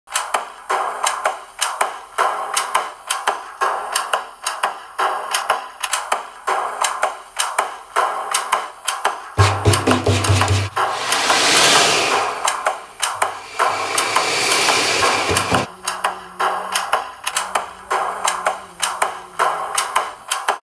Cadeson zvuk oceánu
hudebninyvacha.cz
Profesionální velmi kvalitní buben Cadeson - Zvuk oceánu. Z řady Orfovy nástroje. Vhodné i pro hudební terapii.